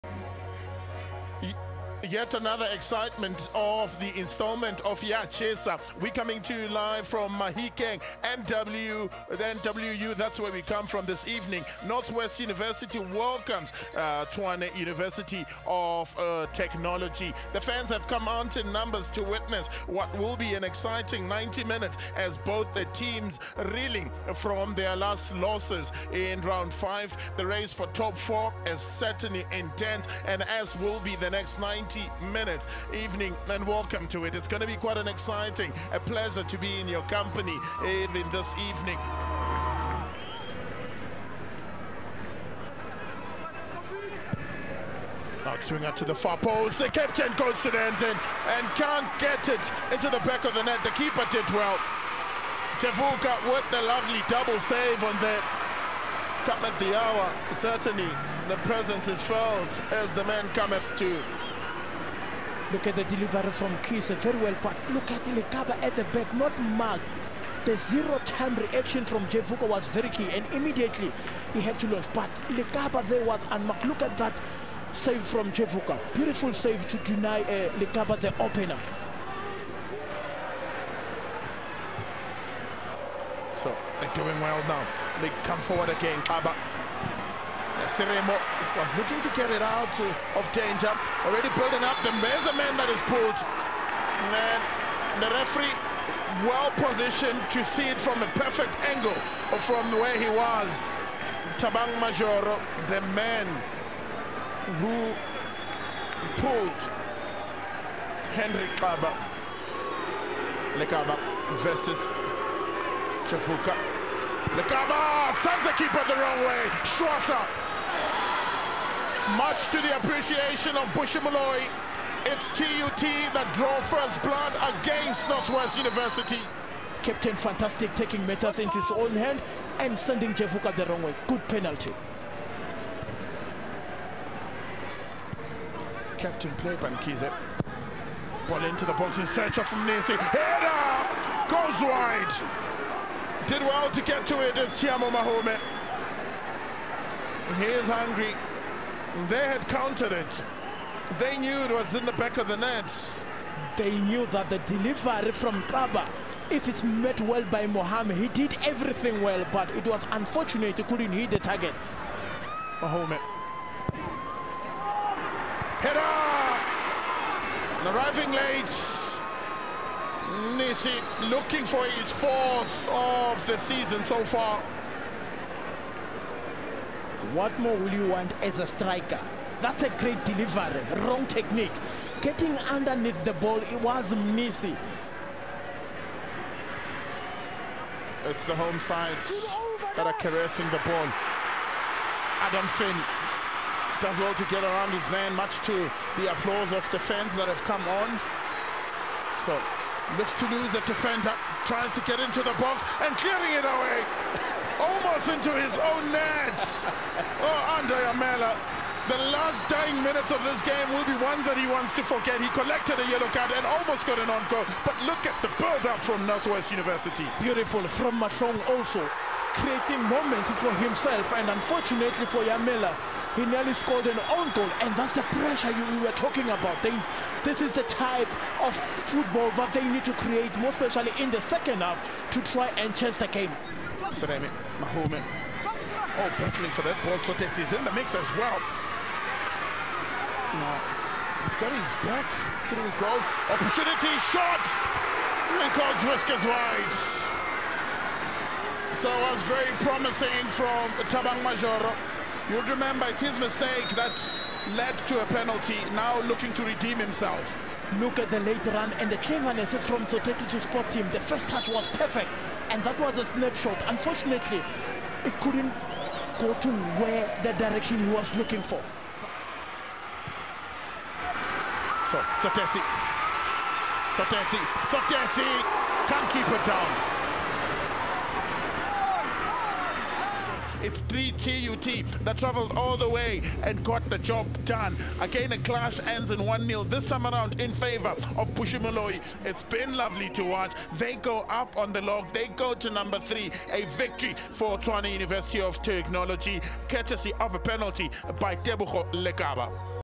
HIGHLIGHTS: NWU vs TUT - round 6
🎥 WATCH highlights of the NWU vs TUT round 6 game where the red army picked up a crucial away victory to keep their semi-final hopes alive.